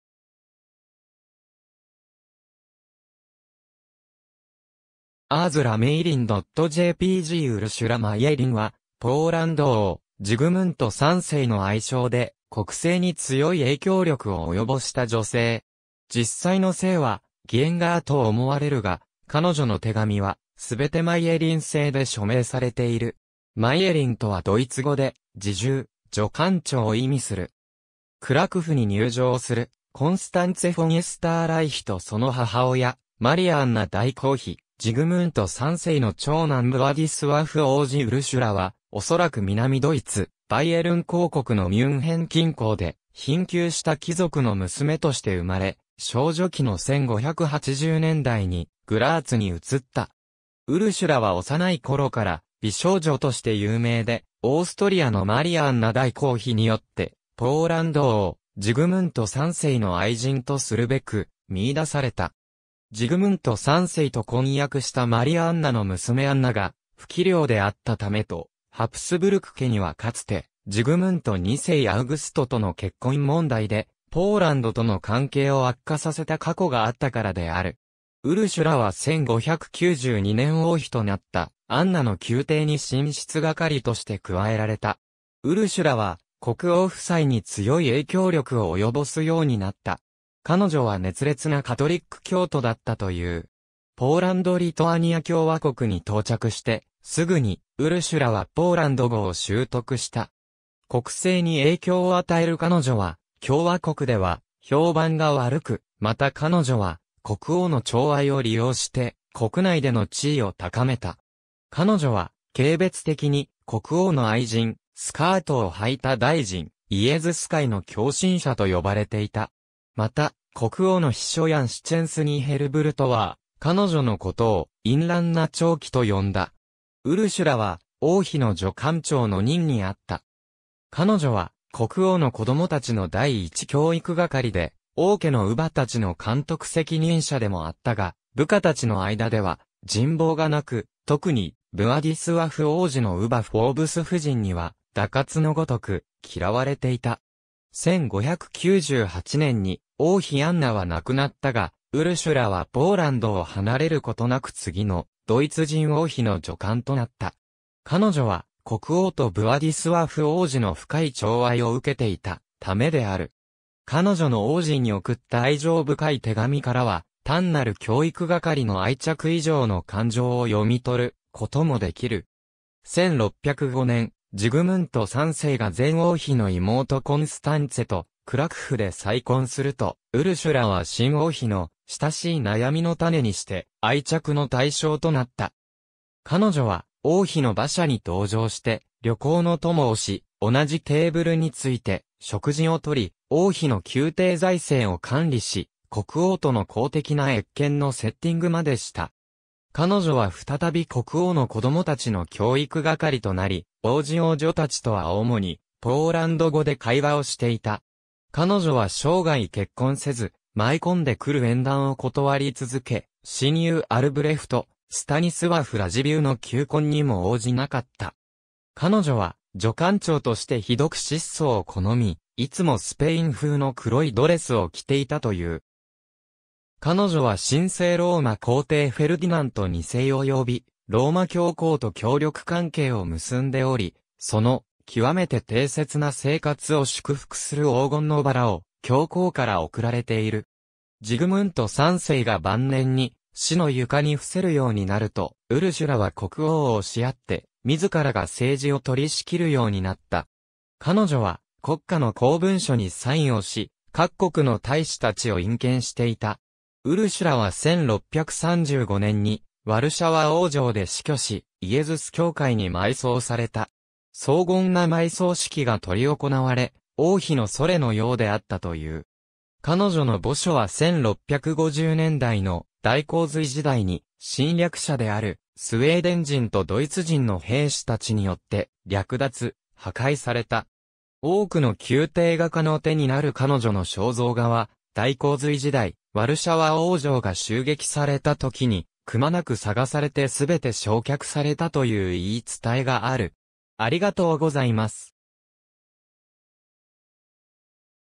ウルシュラ・マイェリン
ポーランド・リトアニア共和国の人物
Urszula Meyerin.jpg ウルシュラ・マイェリン（ポーランド語表記：Urszula Mayerin / ドイツ語表記：Ursula Meierin, 1570年 – 1635年）は、ポーランド王ジグムント3世の愛妾で、国政に強い影響力を及ぼした女性。
実際の姓はギエンガー (Gienger) と思われるが、彼女の手紙は全てマイェリン姓で署名されている。
マイェリン (meierin) とはドイツ語で「侍従、女官長」を意味する。
クラクフに入城するコンスタンツェ・フォン・エスターライヒとその母親マリア・アンナ大公妃（1605年頃） ジグムント3世の長男ヴワディスワフ王子（1605年頃） ウルシュラはおそらく南ドイツ、バイエルン公国のミュンヘン近郊で、貧窮した貴族の娘として生まれ、少女期の1580年代にグラーツに移った。
ウルシュラは幼い頃から美少女として有名で、オーストリアのマリア・アンナ大公妃（カール大公の妻）によってポーランド王ジグムント3世の愛人とするべく見出された。
ジグムント3世と婚約したマリア・アンナの娘アンナが不器量であったためと、ハプスブルク家にはかつてジグムント2世アウグストとの結婚問題でポーランドとの関係を悪化させた過去があったからである。
ウルシュラは1592年王妃となったアンナの宮廷に寝室係として加えられた。
ウルシュラは国王夫妻に強い影響力を及ぼすようになった。
彼女は熱烈なカトリック教徒だったという。
ポーランド・リトアニア共和国に到着してすぐに、ウルシュラはポーランド語を習得した。
国政に影響を与える彼女は共和国では評判が悪く、また彼女は国王の寵愛を利用して国内での地位を高めた。
彼女は軽蔑的に「国王の愛人、スカートを穿いた大臣、イエズス会の狂信者」と呼ばれていた。
また国王の秘書ヤン・シチェンスニィ・ヘルブルトは彼女のことを「淫乱な寵姫」と呼んだ。
ウルシュラは王妃の女官長（Ochmistrzyni）の任にあった。
彼女は国王の子供たちの第一教育係で、王家の乳母たちの監督責任者でもあったが、部下たちの間では人望が無く、特にヴワディスワフ王子の乳母フォーブス夫人（スコットランド人でプロテスタント）には蛇蠍のごとく嫌われていた。
1598年に王妃アンナは亡くなったが、ウルシュラはポーランドを離れることなく次のドイツ人王妃の女官となった。
彼女は国王とヴワディスワフ王子の深い寵愛を受けていたためである。
彼女の王子に送った愛情深い手紙からは、単なる教育係の愛着以上の感情を読み取ることも出来る。
1605年、ジグムント3世が前王妃の妹コンスタンツェとクラクフで再婚すると、ウルシュラは新王妃の「親しい悩みの種にして愛着の対象」となった。
彼女は王妃の馬車に同乗して旅行の供をし、同じテーブルについて食事を摂り、王妃の宮廷財政を管理し、国王との公的な謁見のセッティングまでした。
彼女は再び国王の子供達の教育係となり、王子王女たちとは主にポーランド語で会話をしていた（母親である王妃だけがドイツ語で子供たちと会話していた）。
彼女は生涯結婚せず、舞い込んでくる縁談を断り続け、親友アルブレフト・スタニスワフ・ラジヴィウの求婚にも応じなかった。
彼女は女官長としてひどく質素を好み、いつもスペイン風の黒いドレスを来ていたという。
彼女は神聖ローマ皇帝フェルディナント2世およびローマ教皇と協力関係を結んでおり、その「きわめて貞節な生活」を祝福する黄金の薔薇を教皇から贈られている。
ジグムント3世が晩年に死の床に臥せるようになると、ウルシュラは国王を押しやって自らが政治を取り仕切るようになった。
彼女は国家の公文書にサインをし、各国の大使達を引見していた。
ウルシュラは1635年にワルシャワ王城で死去し、イエズス教会に埋葬された。
荘厳な埋葬式が執り行われ、王妃のそれのようであったという。
彼女の墓所は1650年代の大洪水時代に、侵略者であるスウェーデン人とドイツ人の兵士達によって略奪・破壊された。
多くの宮廷画家の手になる彼女の肖像画は大洪水時代、ワルシャワ王城が襲撃された時に、くまなく探されて全て焼却されたという言い伝えがある。